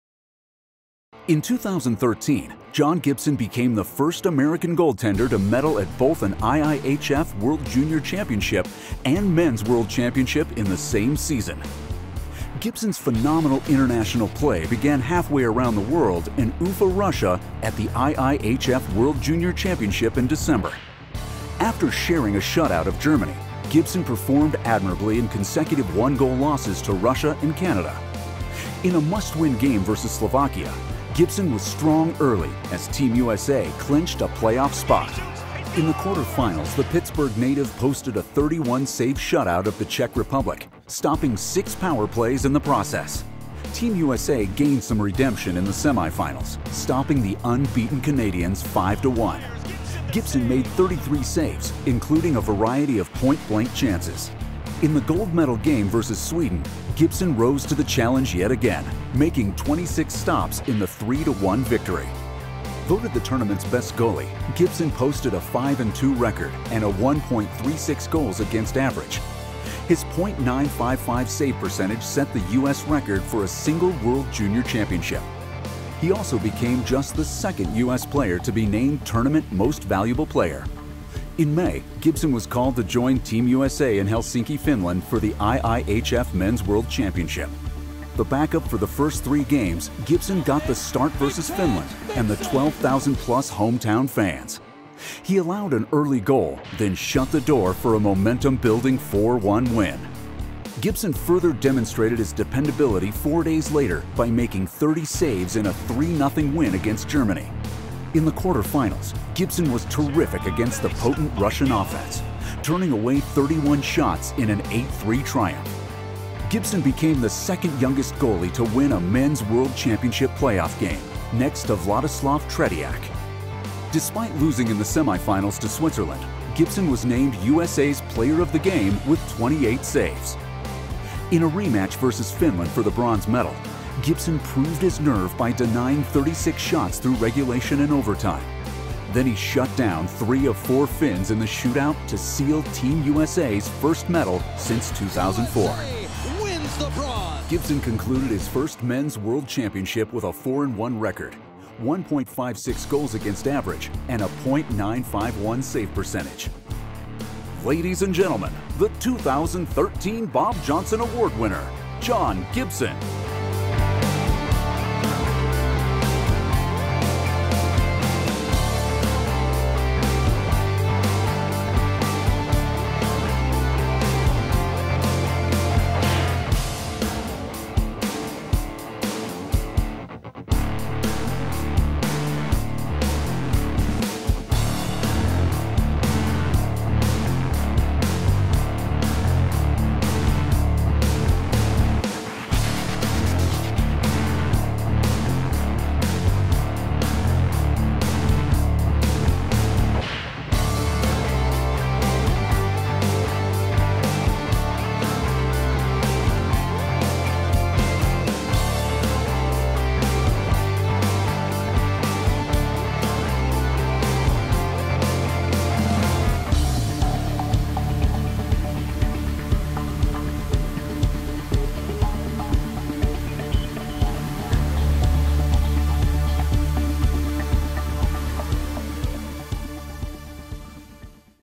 2013 Bob Johnson Award - John Gibson (Highlight Video)
John Gibson takes home the 2013 Bob Johnson Award at USA Hockey's annual President's Award Dinner. Check out his tribute video.